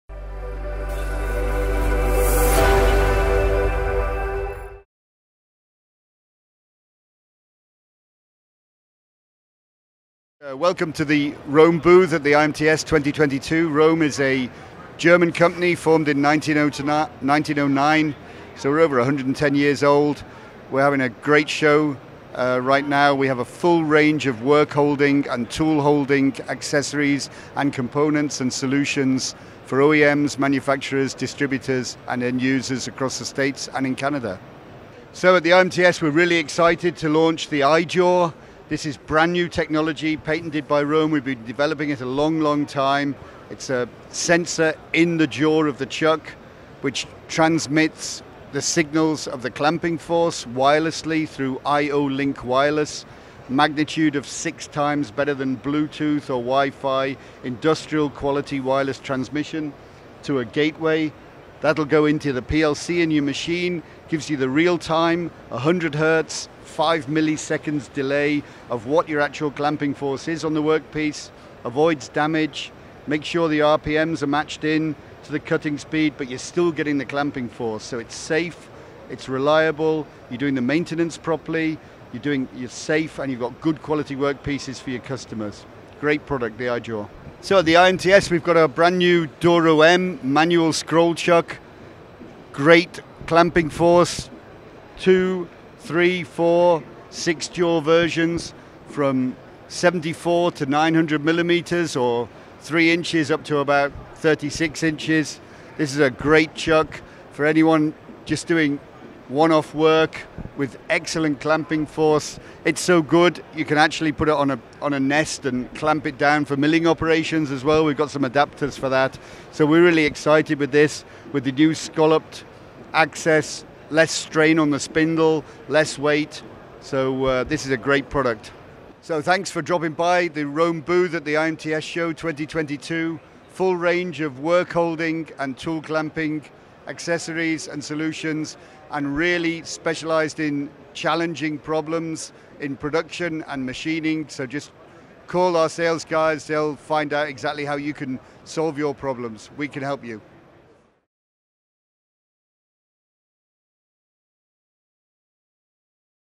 RÖHM at the IMTS 2022
Did you miss the IMTS? Not a problem!

👉 Check out this insightful interview David Jackson had in our booth at IMTS about all the new & exciting we have going on at new products that are launching to find solutions for all your turning, milling, and workholding needs!